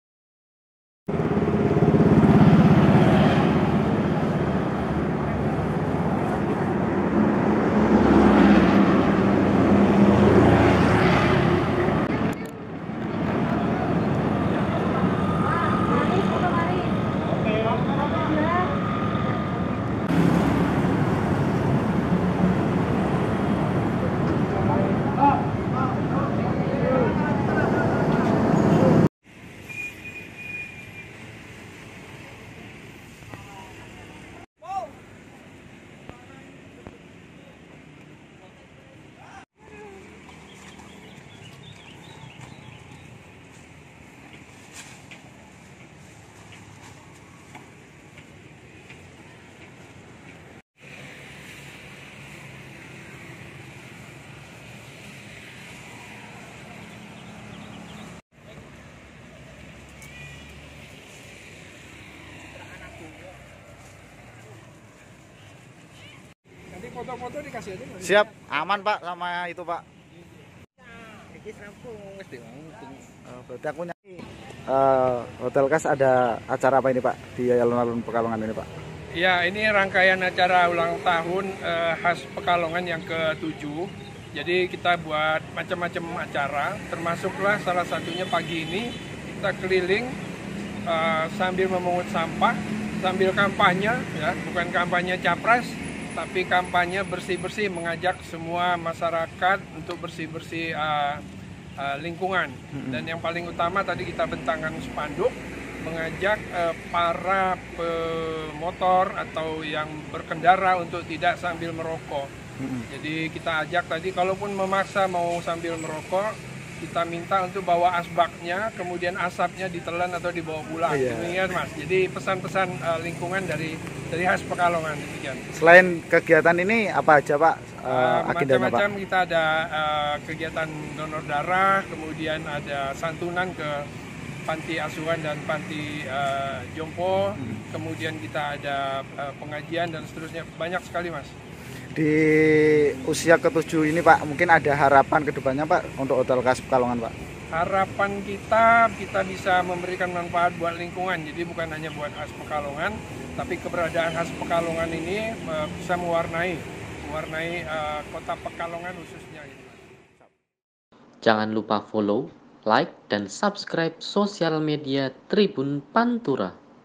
Karyawan Hotel KHAS Pekalongan Pungut Sampah di Jalan dan Sosialisasi Tertib Berlalulintas
Puluhan karyawan Hotel KHAS Pekalongan, gelar kegiatan jalan sehat bersama sembari memungut sampah yang ada di jalan, serta sosialisasi tertib berlalulintas.

Kegiatan ini langsung diikuti oleh GM Hotel KHAS Pekalongan Saunan Rasyid, bersama karyawan, dan bekerjasama sama dengan Satlantas Polres Pekalongan Kota.

Jalan sehat dan pungut sampah di jalan, dimulai dari hotel, menunju ke Jalan Agus Salim, lalu belok ke Jalan Bandung, kemudian melewati Jalan Hasanuddin, dan finis di Alun-alun Kota Pekalongan.

Reporter: Indra Dwi Purnomo
Editor: Galih